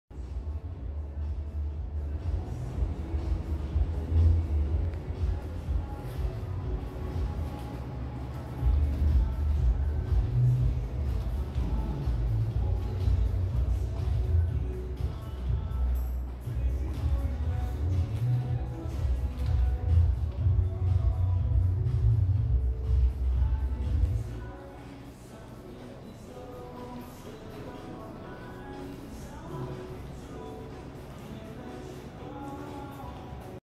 [서울촌놈] 촬영지 !!! 부산 섬에서 가장 핫 플레이스 중 한 곳?
요즘 가장 핫 하다는 영도 카린플레이스
게스트 룸을 소개합니다.